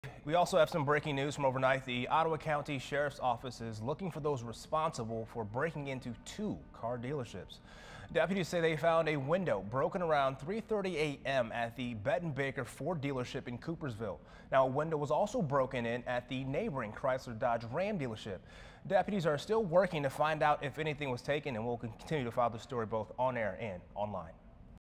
Two Coopersville car dealerships broken into overnight
The Ottawa County Sheriff's Office is investigating after two car dealerships in Coopersville were broken into overnight Tuesday.

According to a press release, around 3:25 a.m. on Tuesday, a deputy was on patrol and found a broken window at the Betten Baker Ford dealership, located at 940 O'Malley Drive.

While investigating, deputies determined that unknown suspects broke a glass window in order to get inside the dealership. Suspects also broke a window at the Chrysler/Dodge/Ram dealership next door.

Deputies are currently working with dealership employments to determine what, if anything, was taken from either business.